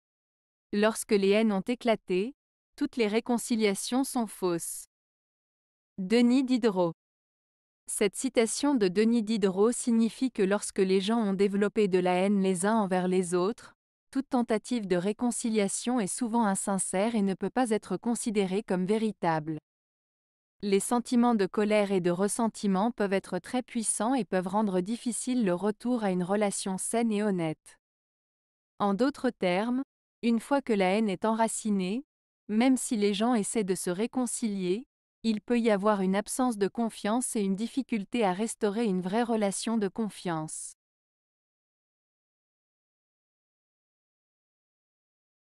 Lorsque les haines ont éclaté, toutes les réconciliations sont fausses. - Denis Diderot
Cette citation de Denis Diderot signifie que lorsque les gens ont développé de la haine les uns envers les autres, toute tentative de réconciliation est souvent insincère et ne peut pas être considérée comme véritable. Les sentiments de colère et de ressentiment peuvent être très puissants et peuvent rendre difficile le retour à une relation saine et honnête. En d'autres termes, une fois que la haine est enracinée, même si les gens essaient de se réconcilier, il peut y avoir une absence de confiance et une difficulté à restaurer une vraie relation de confiance.